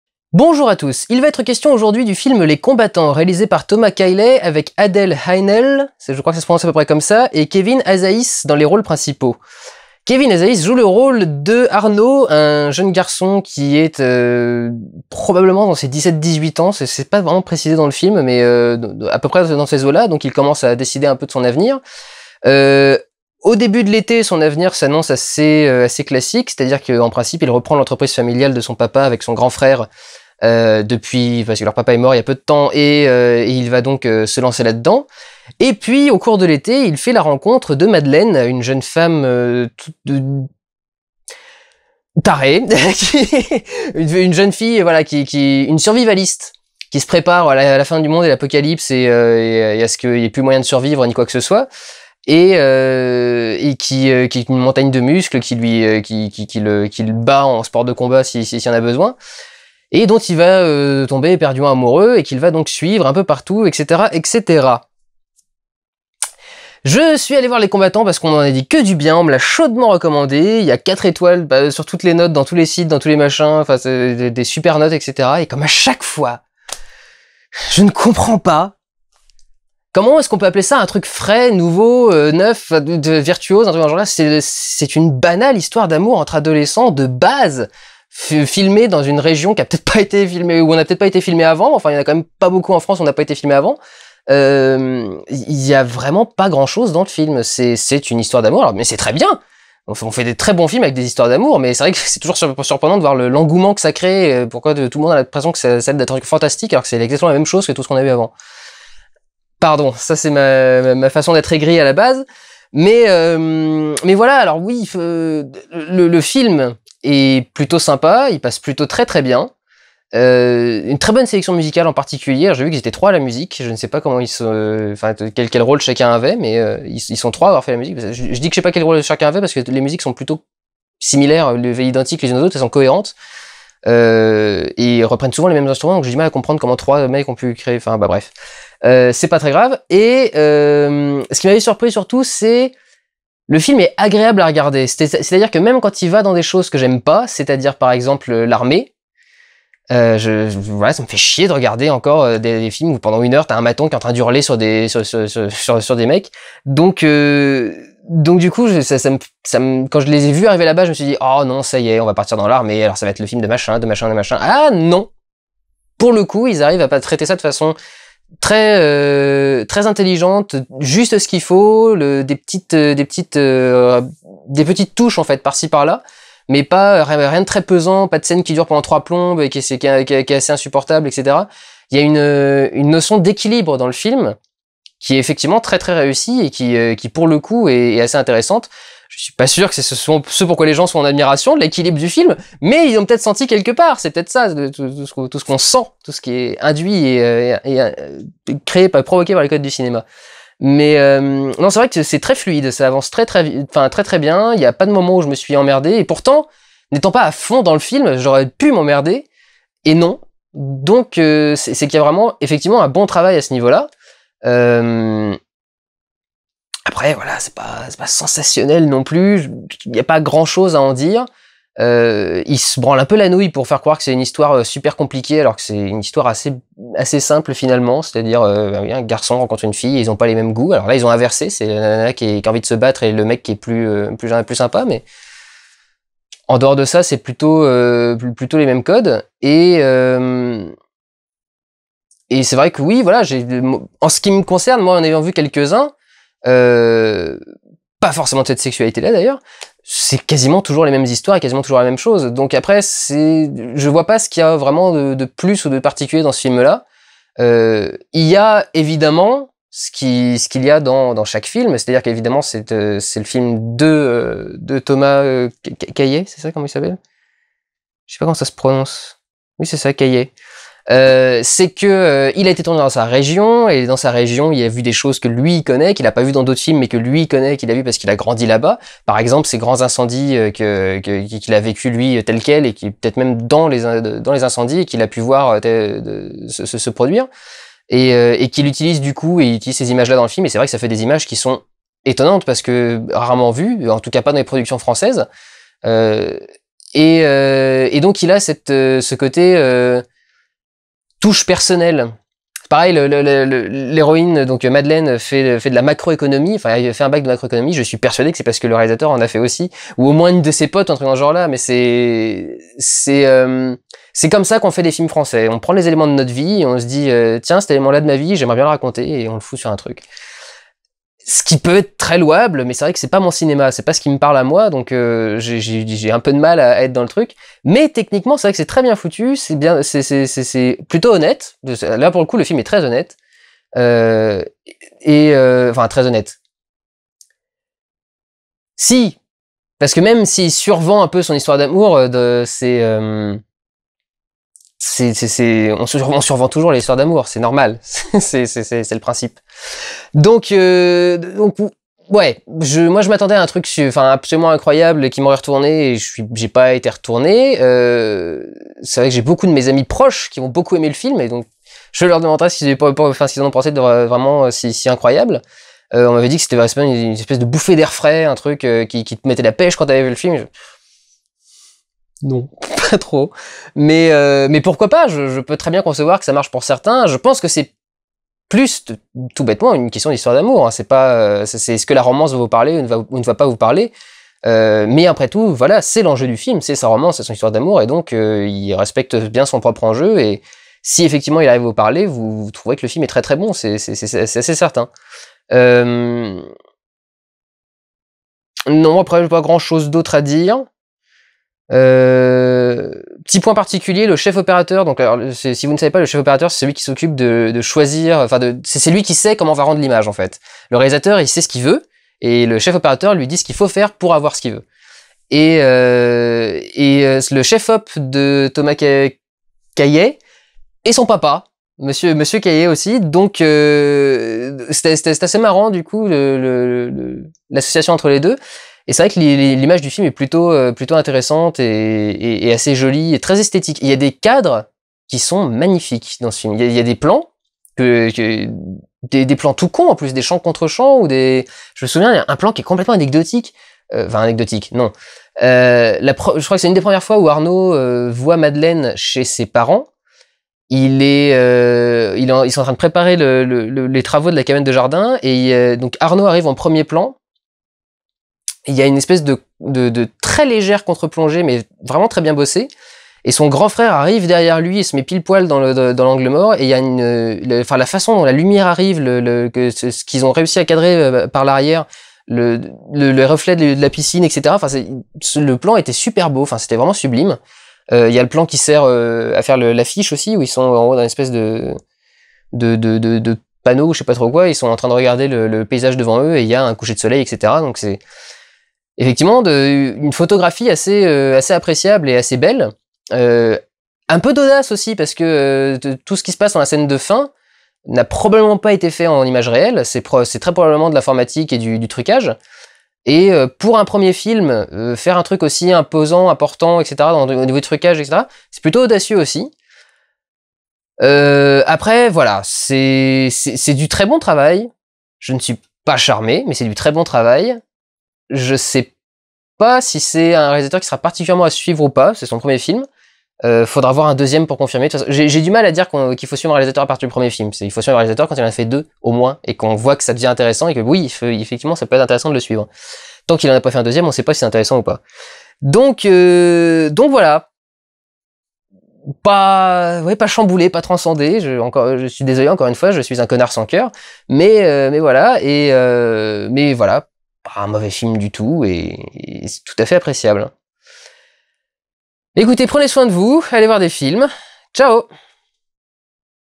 Vlog - Les Combattants
Mon avis sur Les Combattants, réalisé par Thomas Cailley.